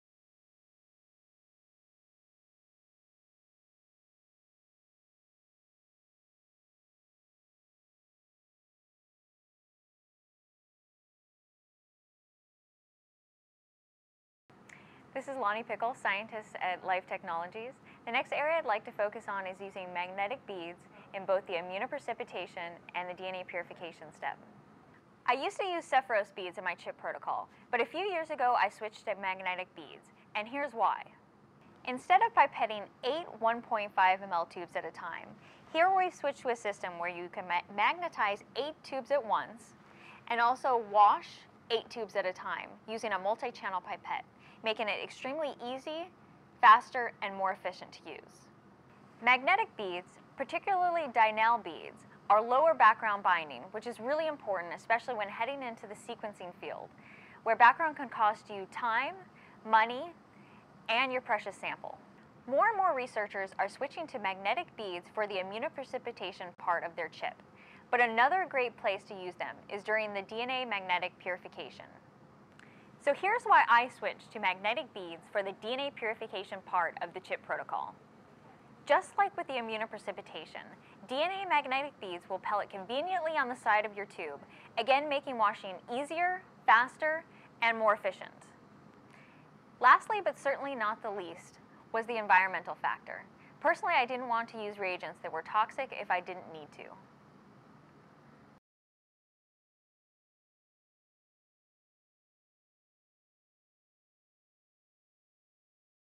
ChIP Optimization: Using Magnetic Beads in ChIP
Loni Pickle, Life Technologies R&D Scientist, highlights some of the benefits of using magnetic beads in chromatin immunoprecipitation (ChIP) sample processing.

Tissue Processing Using Magnetic Beads -- Shearing Optimization

[audio transcript]

This is Loni Pickle, Scientist at Life Technologies; the next area I'd like to focus on is using magnetic beads in both the immunoprecipitation and DNA purification step.

I used to use sepharose beads in my ChIP protocol, but a few years ago I switched to magnetic beads and here's why:

Instead of pipetting eight 1.5 ml tubes at a time, here we switch to a system where you can magnetize 8 tubes at once, and also wash 8 tubes at a time using a multi-channel pipette, making it extremely easy, faster and more efficient to use.

Magnetic beads - particularly Dynabeads - are lower background binding, which is really important especially when heading into the sequencing field where background can cost you time, money, and your precious sample.

More and more researchers are switching to magnetic beads for the immunoprecipitation part of their ChIP, but another great place to use them is during the DNA magnetic purification.

So here's why I switched to magnetic beads for the DNA purification part of the ChIP protocol: Just like with immunoprecipitation, DNA magnetic beads will pellet conveniently on the side of your tube; again making washing easier, faster, and more efficient.

Lastly, but certainly not the least, was the environmental factor: Personally I didn't want to use reagents that were toxic if I didn't need to.